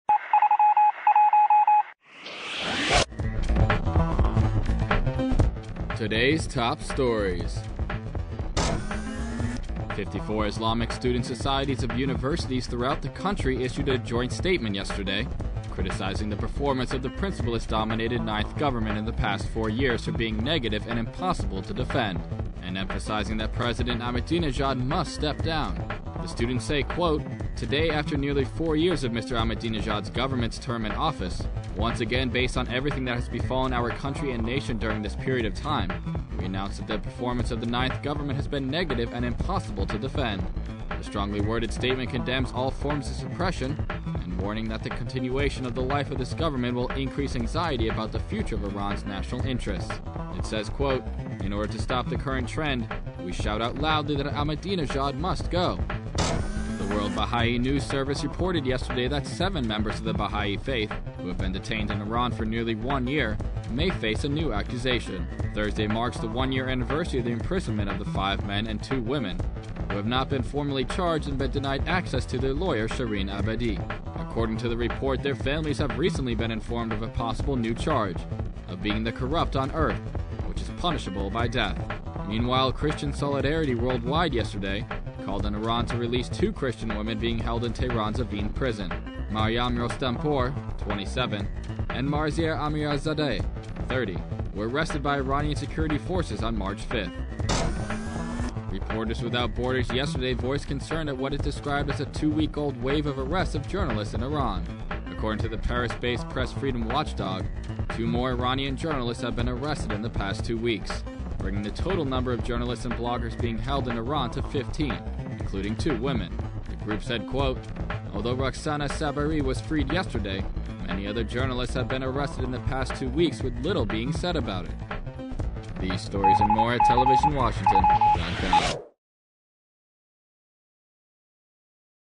Today's Top Stories (May 13, 2009)
Islamic student societies:  Ahmadinejad must go
Fifty four Islamic student societies of universities throughout the country issued a joint statement yesterday, criticizing the performance of the principle-ist-dominated ninth government in the past four years for being negative and impossible to defend and emphasizing that President Mahmoud Ahmadinejad must step down.
The students say: Today, after nearly four years of Mr Ahmadinejads governments term in office, once again, based on everything that has befallen our country and nation during this period of time, we announce that the performance of the ninth government has been negative and impossible to defend.
The strongly-worded statement condemns all forms of suppression and, warning that the continuation of the life of this government will increase anxiety about the future of Irans national interests, it says:  In order to stop the current trend, we shout out loudly that Ahmadinejad must go.
Seven Bahais in Iran may face new charge
The World Bahai News Service reported yesterday that seven members of the Bahai faith, who have been detained in Iran for nearly one year, may face a new accusation.
Thursday marks the one-year anniversary of the imprisonment of the five men and two women, who have not been formally charged and been denied access to their lawyer, Shirin Ebadi.
According to the report, their families have recently been informed of a possible new charge, of being the corrupt on Earth, which is punishable by death.
Meanwhile, Christian Solidarity Worldwide yesterday called on Iran to release two Christian women being held in Tehrans Evin Prison. Maryam Rostampour, 27, and Marzieh Amirizadeh, 30, were arrested by Iranian security forces on 5 March.
Press freedom group warns of wave of arrests in Iran
Reporters Without Borders yesterday voiced concern at what it described as a two-week-old wave of arrests of journalists in Iran.
According to the Paris-based press freedom watchdog, two more Iranian journalists have been arrested in the past two weeks, bringing the total number of journalists and bloggers being held in Iran to 15, including two women.
The group said: Although Roxana Saberi was freed yesterday, many other journalists have been arrested in the past two weeks with little being said about it.